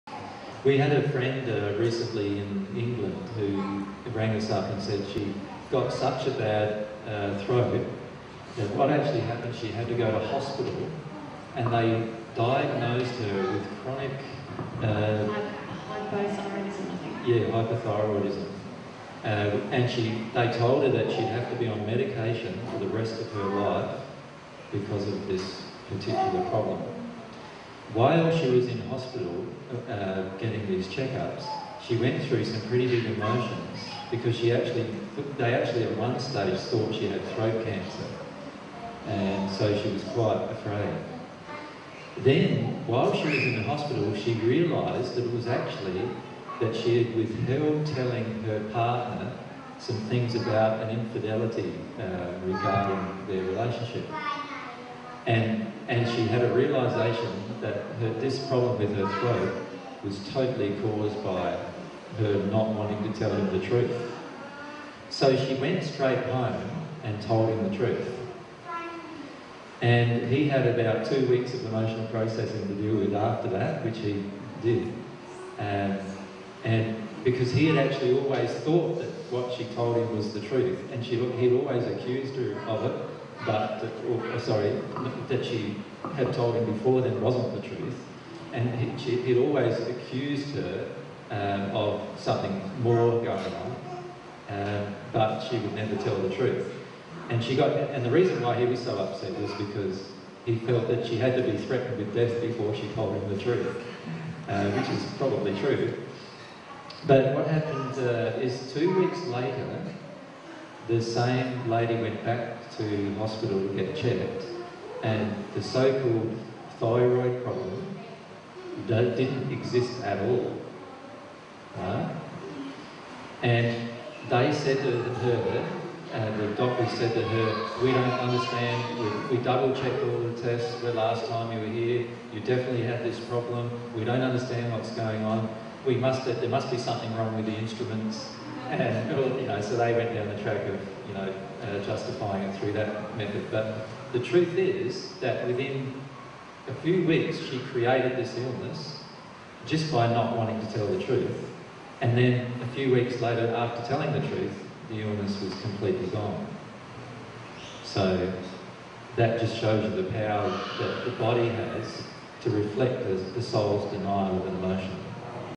Jesus on The Curative Power of Truth
Jesus shares an illustration of the power of truth to cure illness.

This is a section of the presentation by Jesus entitled "20090308 Human Relationships - Parenting Children S2P1".

dtstart:010514:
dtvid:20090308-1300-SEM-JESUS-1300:
dtmd5:6d0ad3bb8abf4778b48fd54607fc28b6: